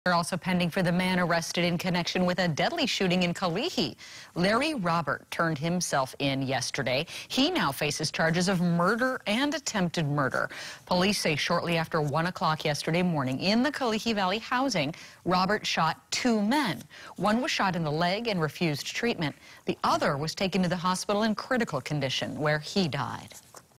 Alleged Kalihi murder suspect turns himself in
Leri Robert turned himself in Monday  in connection with a deadly shooting in Kalihi.